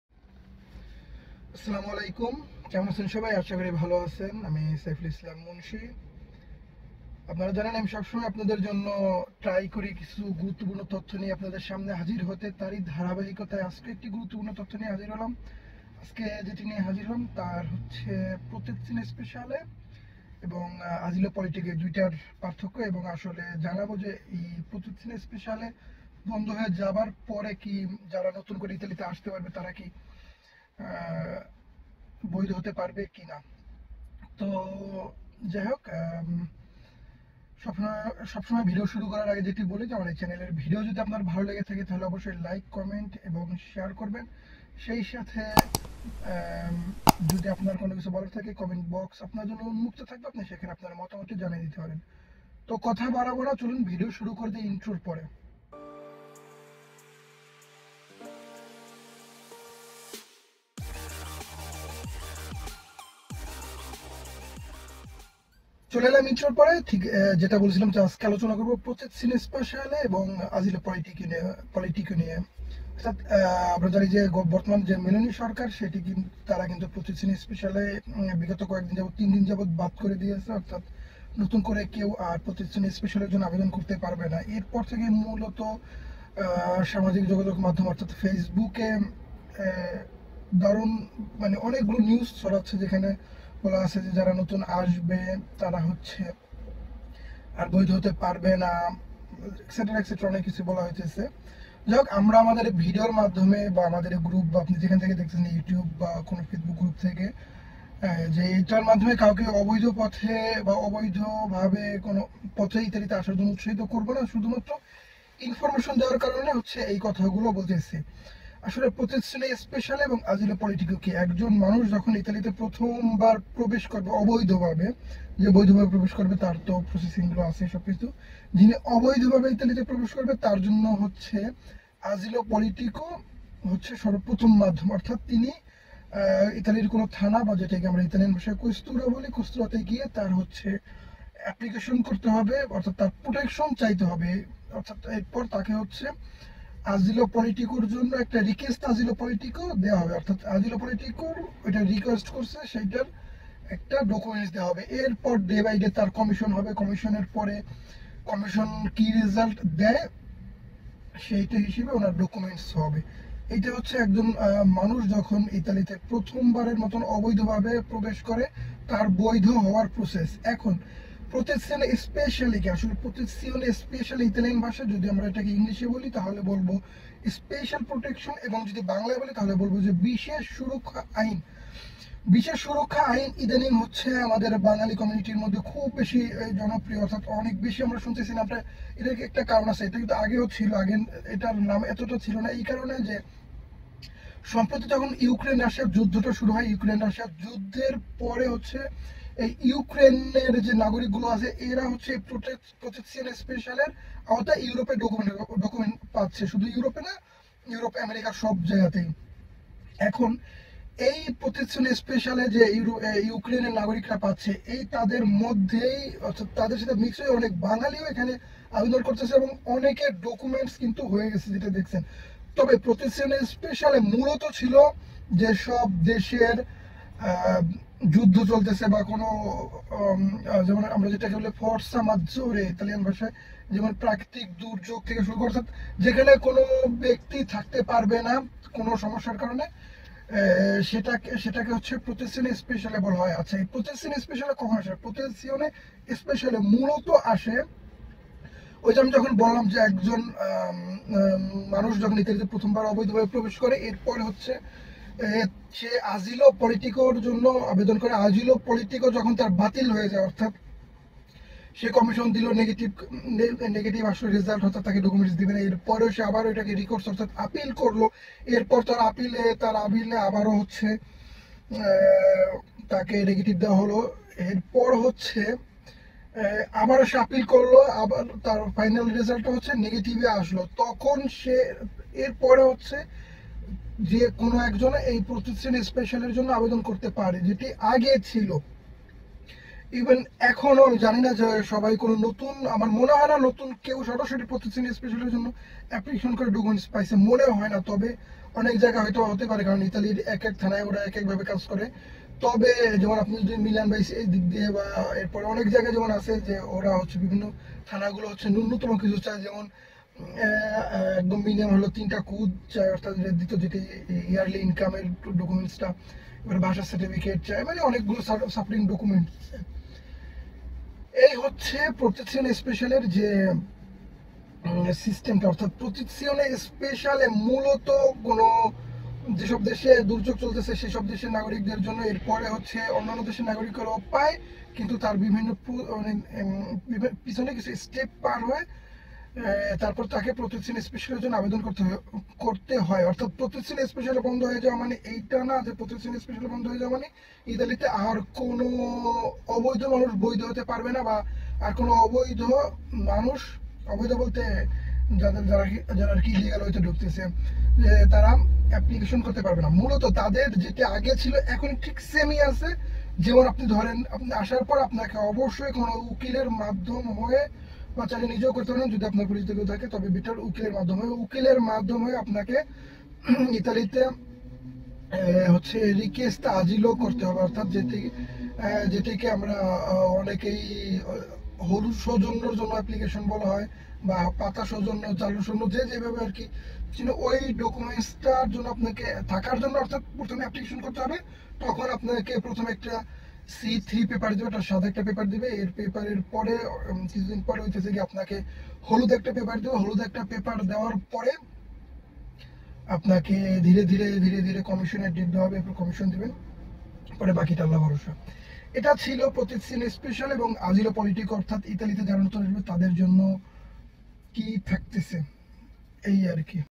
PROTEZIONE SPECIALE বাতিলের পর ইতালিতে কি আর কেউ বৈধ হতে পারবেনা ?
যেকোনো প্রশ্নের জন্য কমেন্ট বক্স উন্মুক্ত